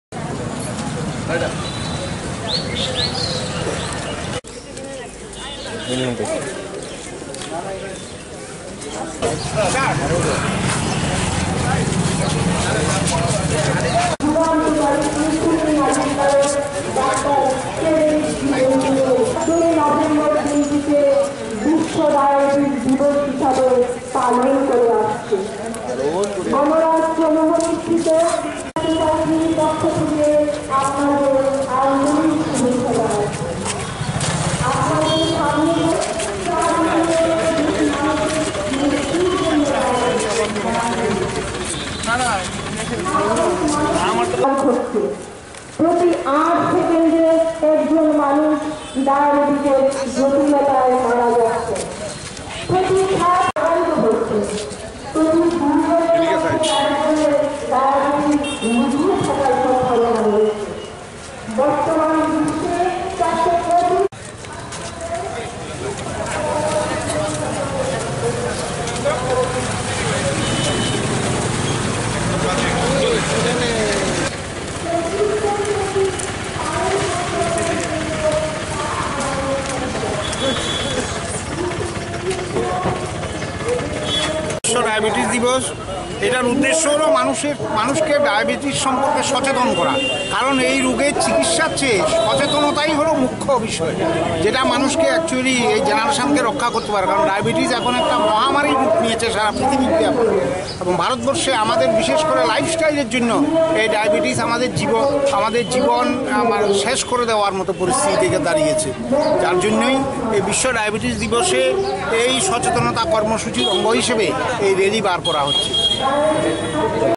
World Diabetes Day Rally jointly organized by Diabetes Welfare Society and H&FW Department
World Diabetes Day Rally jointly organized by Diabetes Welfare Society and Health and Family Welfare Department, Govt of Tripura